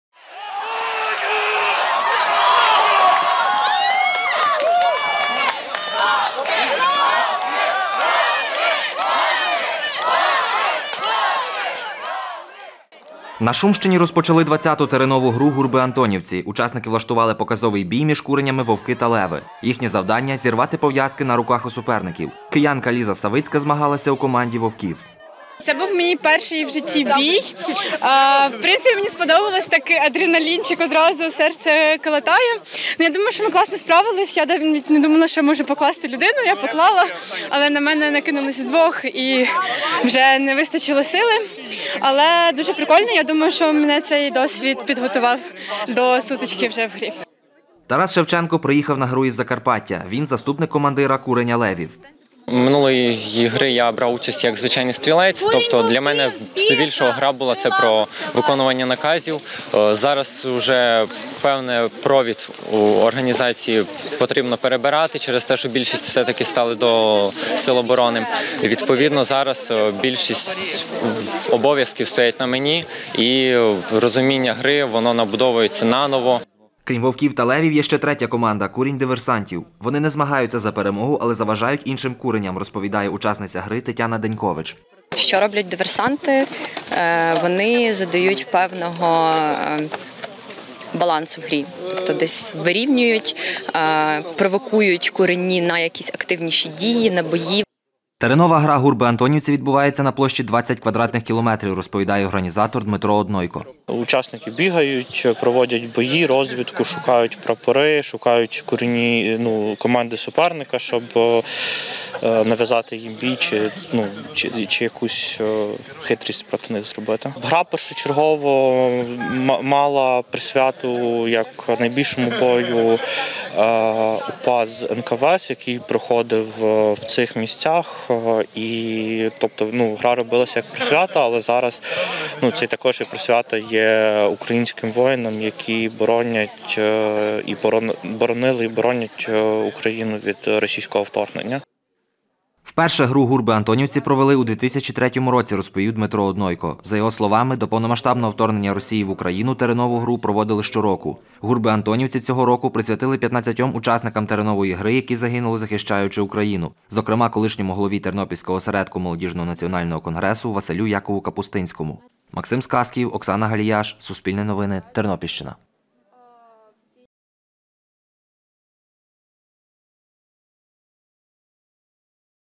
Теренова гра «Гурби-Антонівці» стартувала на Тернопільщині
20-ту теренову гру "Гурби-Антонівці" розпочали на Шумщині. Її проводить громадська організація "Молодіжний націоналістичний конгрес". Цьогоріч у грі беруть участь сто хлопців і дівчат. Гра триватиме до 11 травня. Про це розповів організатор Дмитро Однойко. Гурби-Антонівці